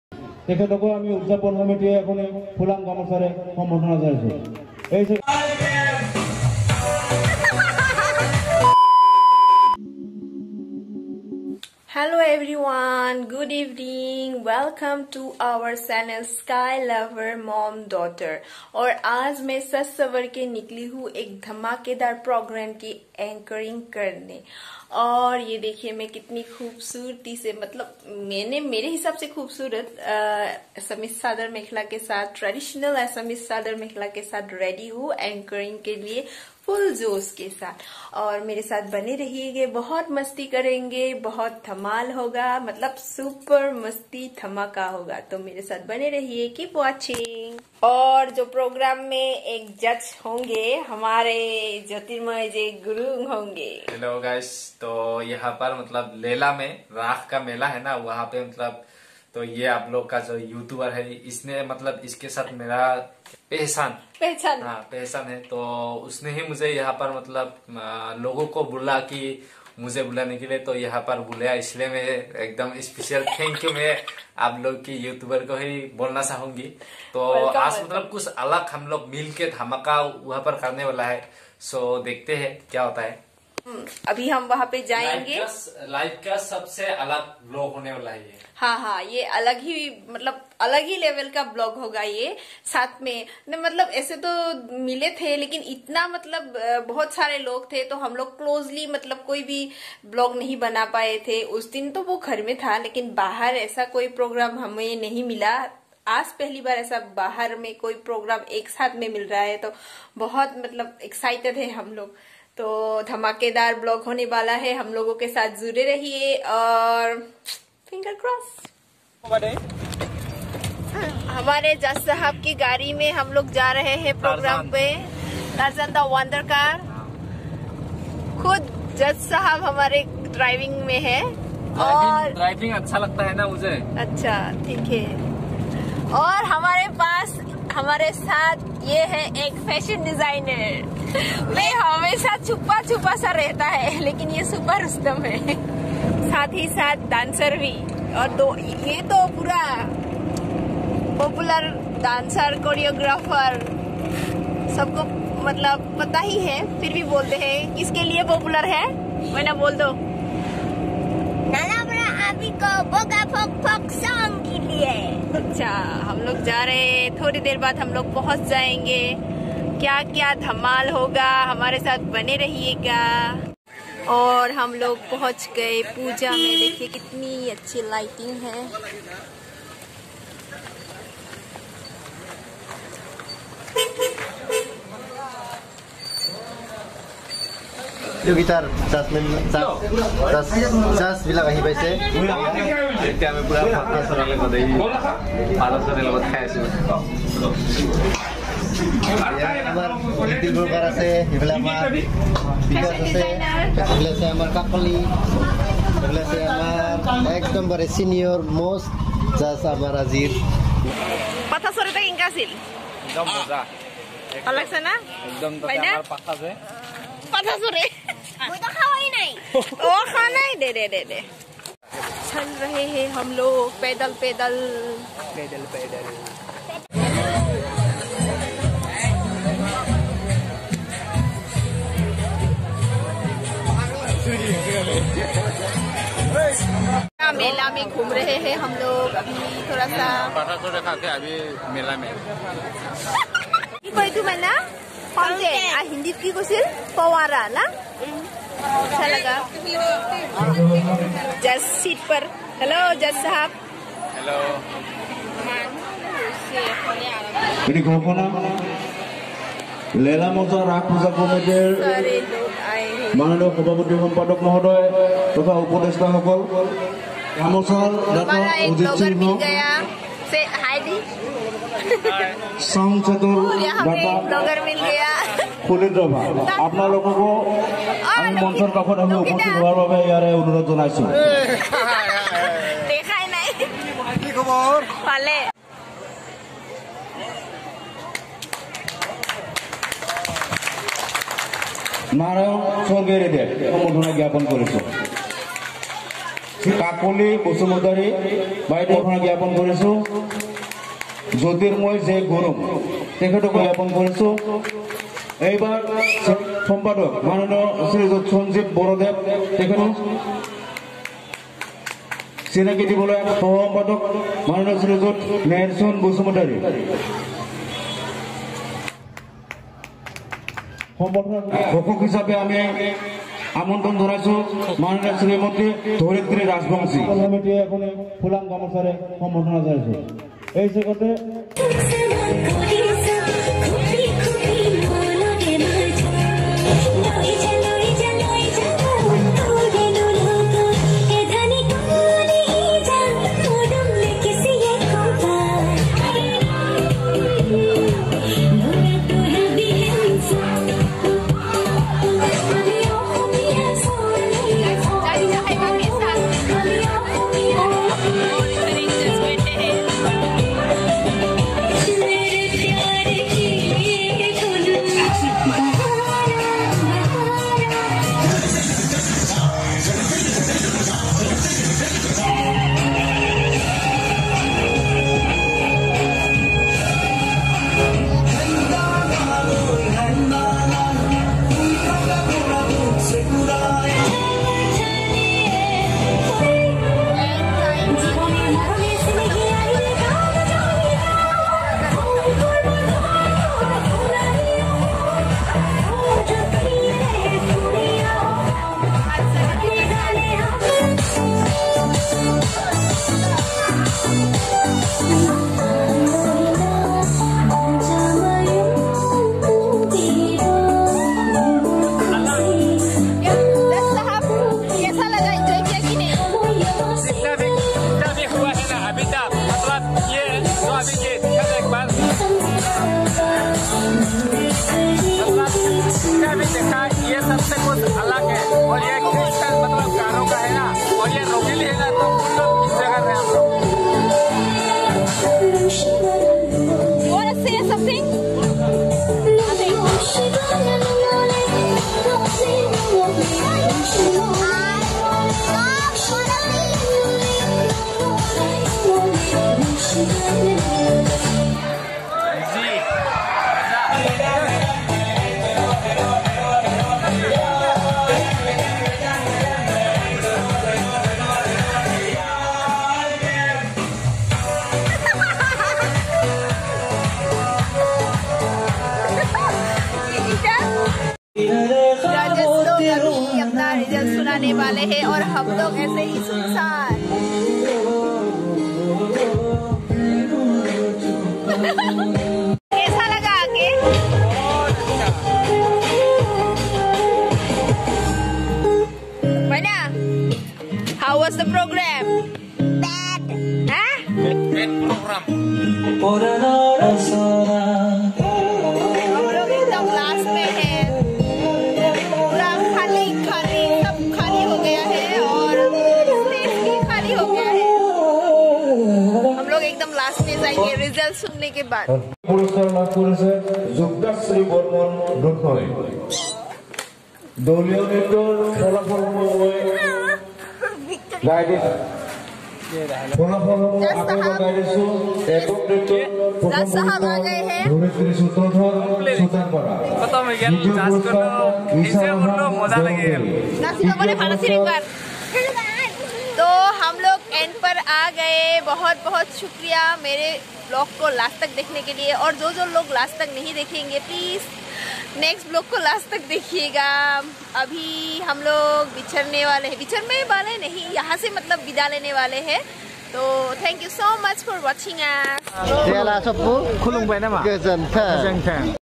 Dhamaka Dance Competition || Anchoring ..miliye do ustadose
Dance Competition at Ras Puja Lela ..

1st Time Hosted a Stage Show Dance Competition..